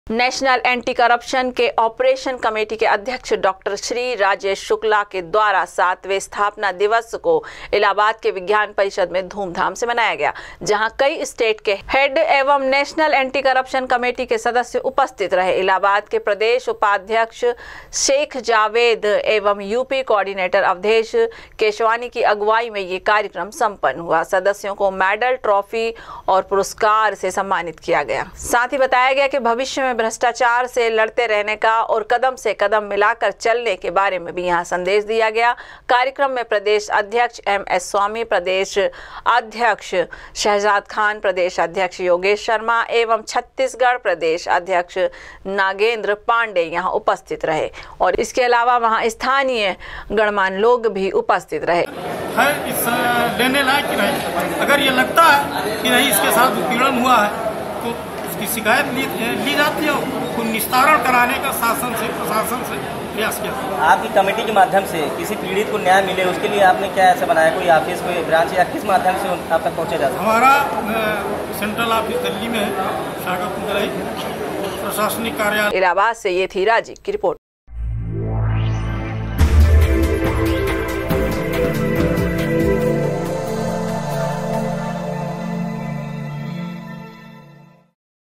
[ Allahabad ] ऑपरेशन कमेटी के अध्यक्ष के द्वारा 7वें स्थापना दिवस को इलाहाबाद में मनाया गया
नेशनल एन्टी करप्शन एंड ऑपरेशन कमेटी के अध्यक्ष डॉ. श्री राजेश शुक्ला जी द्वारा सातवें स्थापना दिवस को इलाहाबाद के विज्ञान परिसद में धूम धाम से मनाया गया